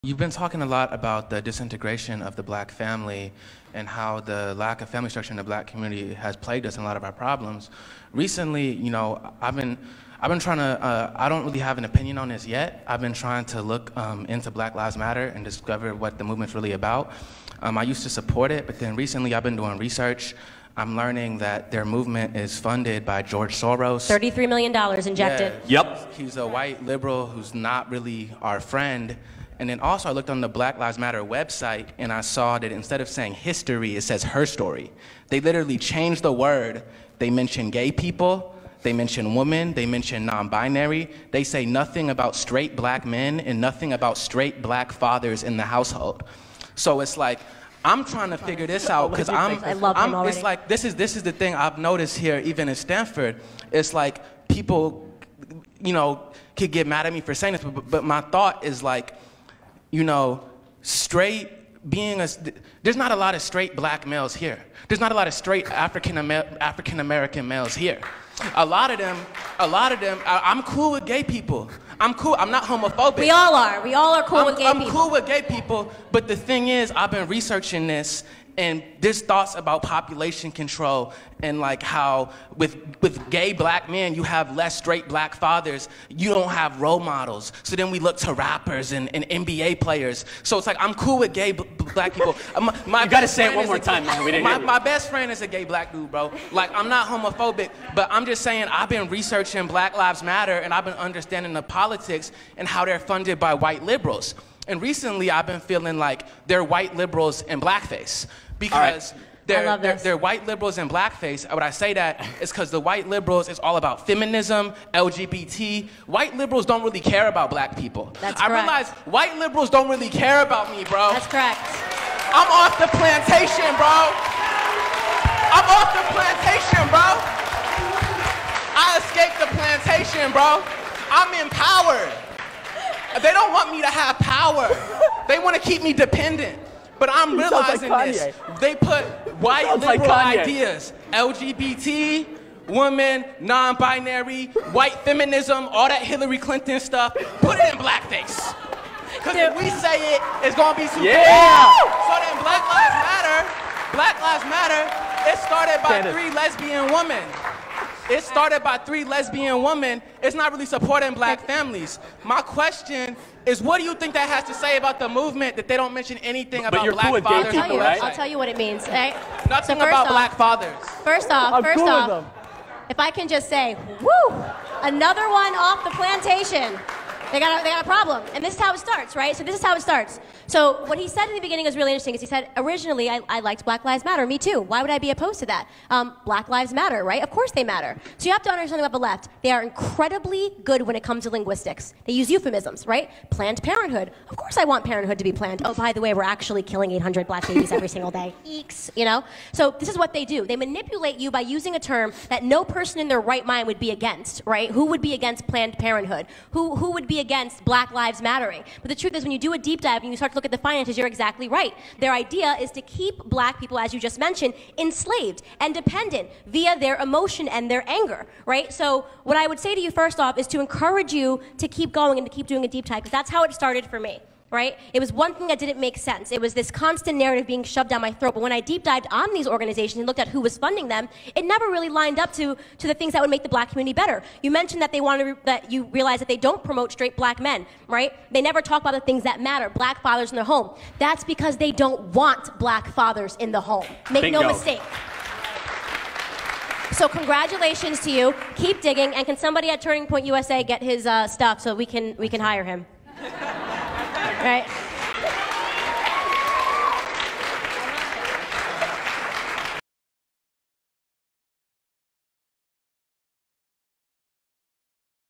BLM is white liberals in blackface
Black student calls out BLM and TPUSA event